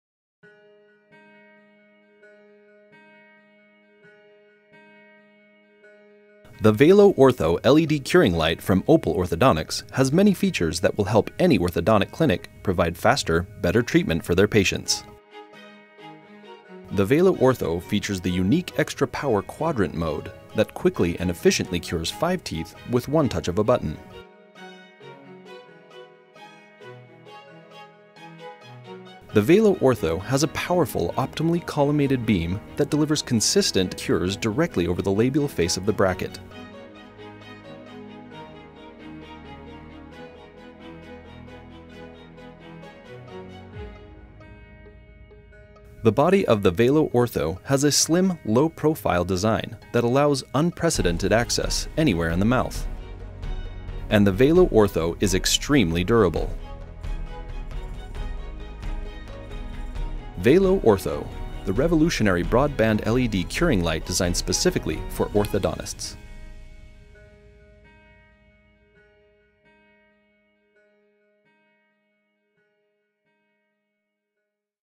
VALO Ortho curing light by Opal Orthodontics
VALO is the ideal light for indirect bonding. Both in the lab and during bonding, VALO’s unique Xtra power mode and convex glass lens offer a powerfully collimated beam that can penetrate the additional material and distance to sufficiently cure brackets.